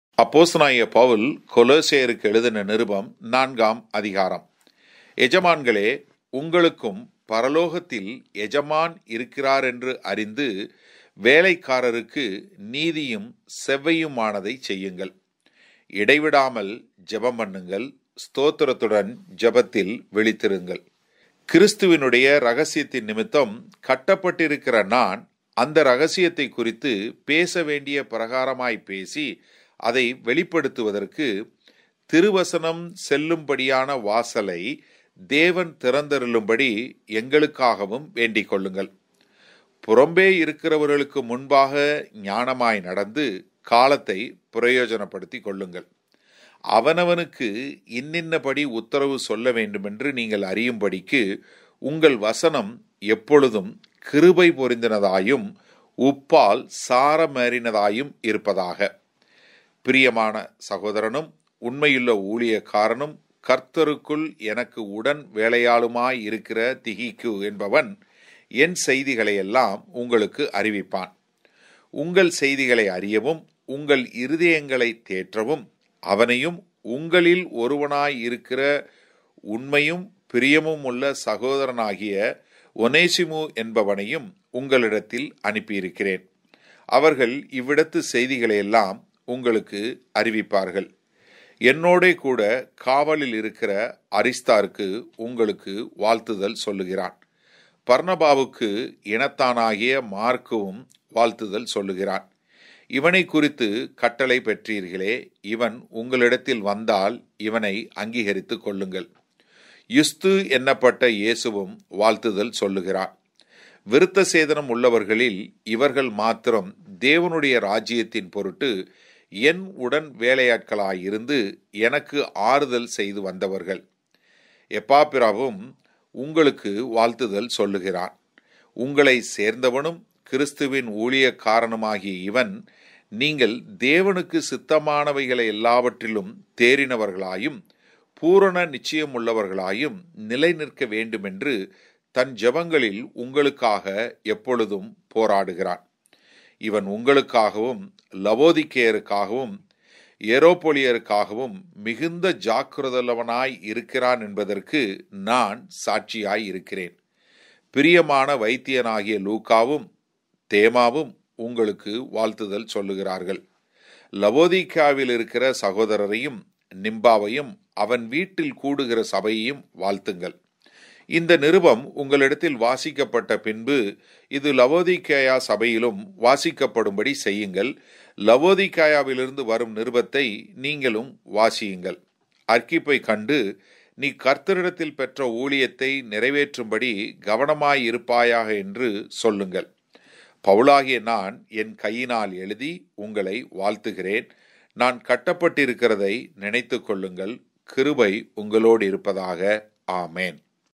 Tamil Audio Bible - கொலோசெயர் அதிகாரம் 04 - Colossians Chapter 04
Tamil Audio Bible read by Bro. Abraham David John, World Tamil Christian Fellowship, London, United Kingdom. Tamil Bible Audio